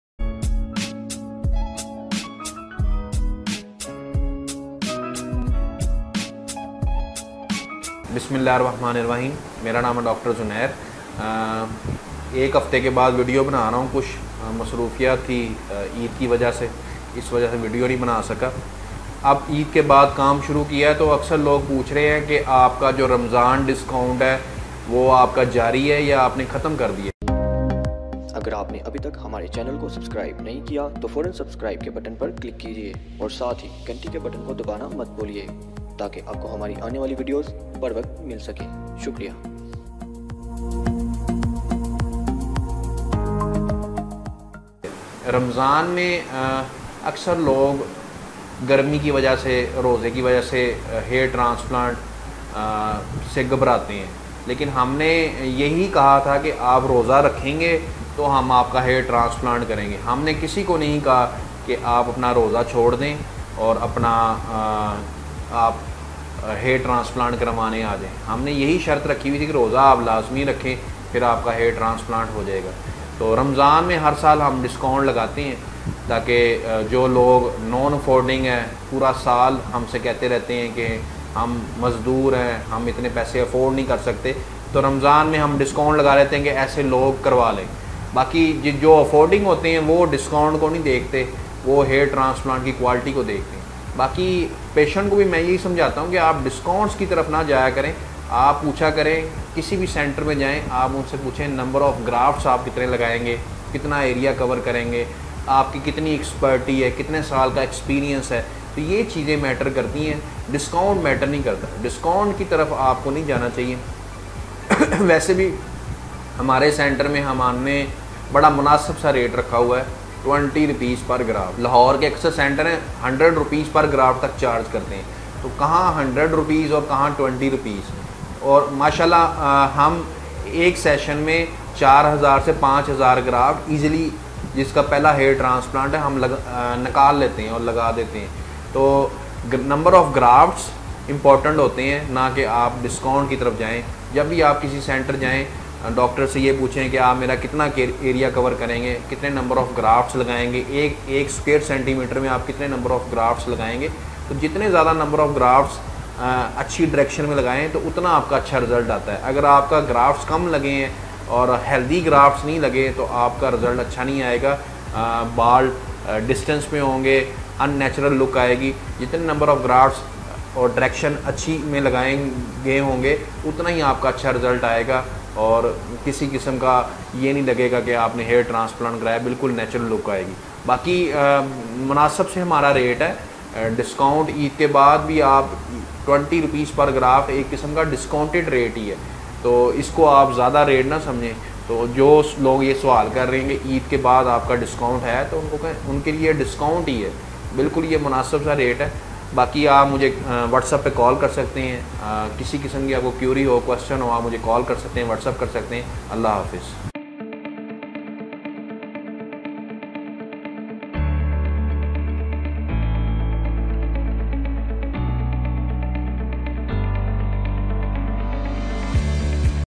What Is The Average Cost Of A Hair Transplant Surgery | Hair Transplant By Dr Zunair Munir
This Video For Hair Transplant surgery by Dr Zunair Muneer.

location : Bilal Chowk Samnabad Faisalabad Pakistan.